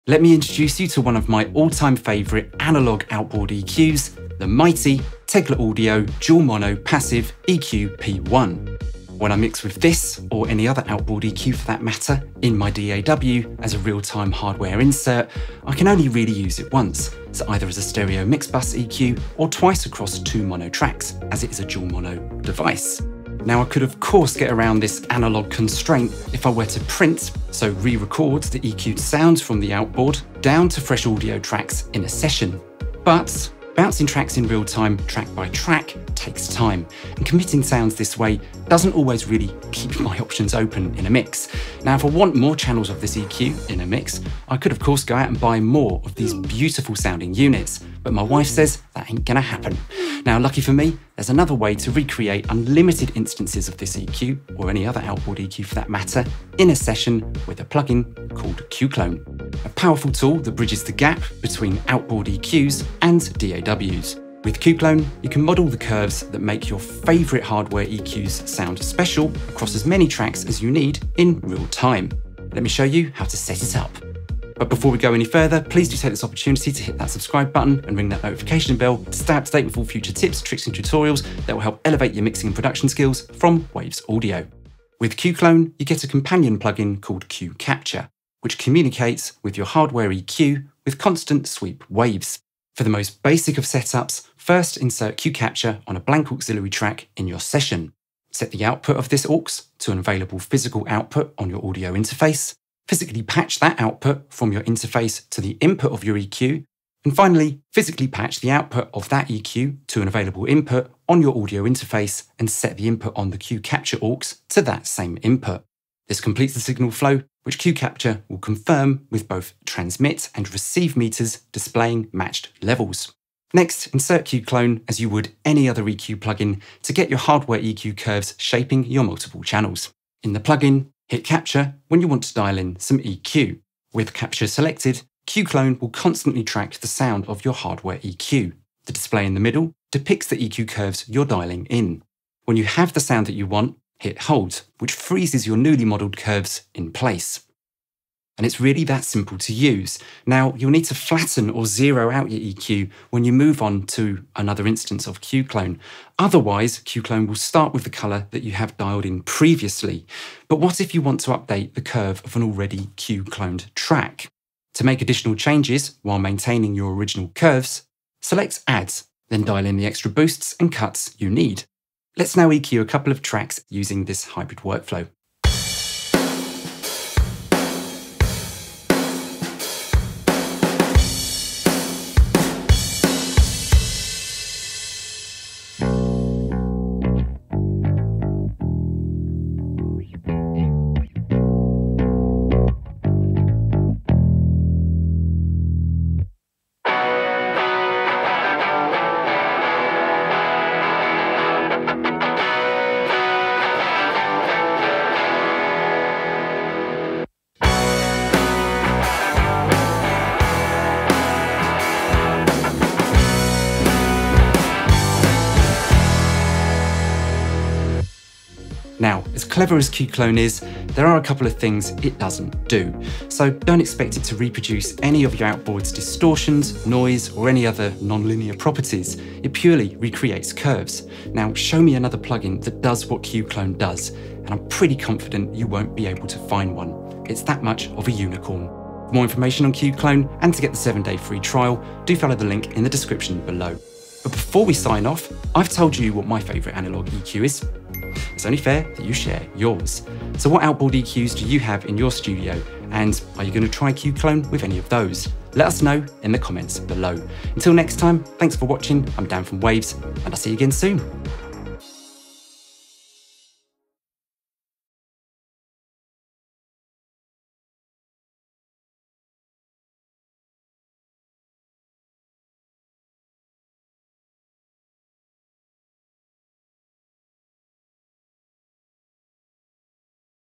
How to Get More Digital Mileage from your Analog EQs
Got some analog EQs lying around but not enough to mix in hybrid? You can! Use Q-Clone – a one-of-a-kind plugin which allows you to get analog tone on infinite channels.

0:00 Introduction
0:49 What is Q-Clone?
1:24 How to setup Q-Clone and Q-Capture in any DAW
2:52 Q-Clone examples
3:37 What Q-Clone doesn’t do
4:04 What are your favorite outboard analog EQs?

If your studio has some outboard analog EQs then you know there’s nothing quite like the warmth and feel these devices deliver in a mix. But, you may only own one unit and cannot afford to buy more. With Waves Q-Clone you have the power to model the curves of your favorite hardware EQs in real-time across as many channels as you need in a session.

From boutique EQs through to guitar-tone stomp boxes, Q-Clone captures it all. In this tutorial, learn how to get setup and running with this unicorn plugin.

Don’t own any hardware EQ? No problem, check out Waves analog model plugins catalog